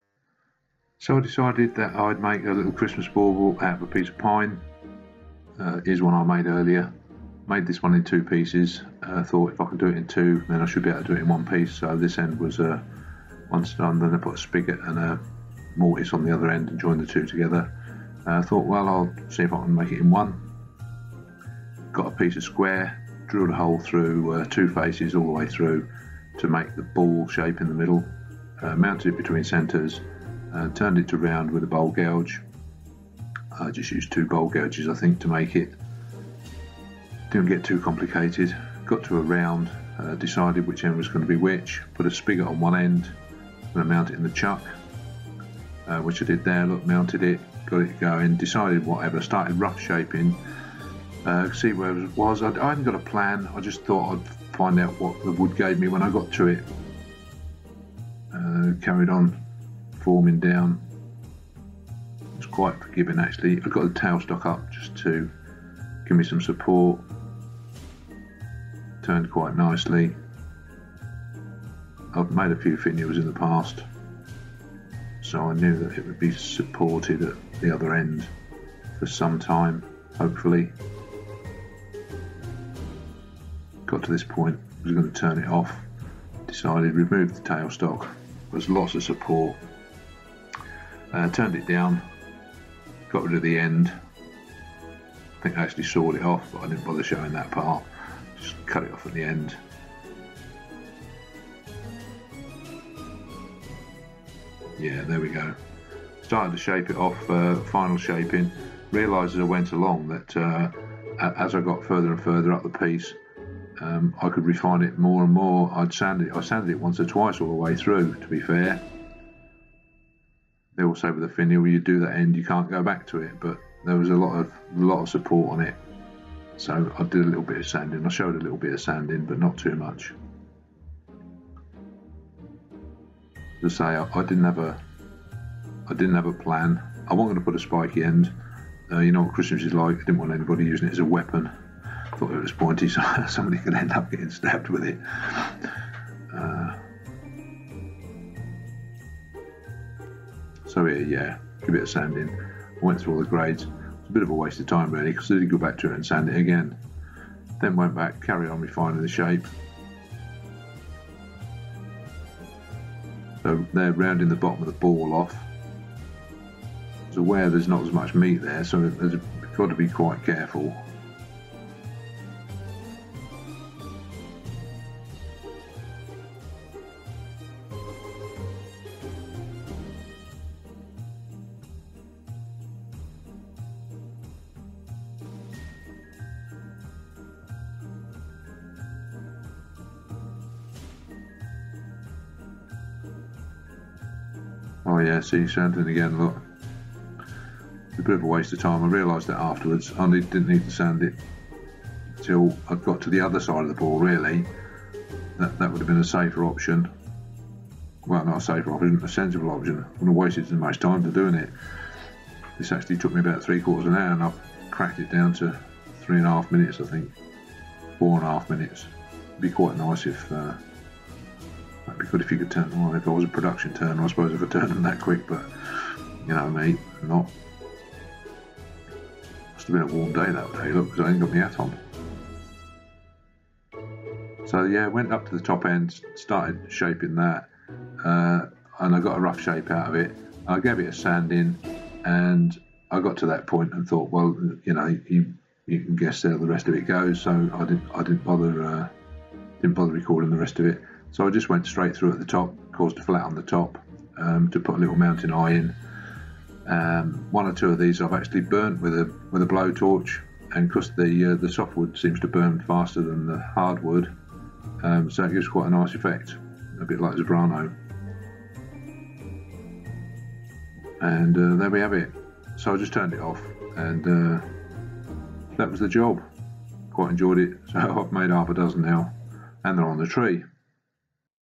Turned Hanging Christmas bauble
A lightweight bauble to practice some spindle turning with a drilled through centre.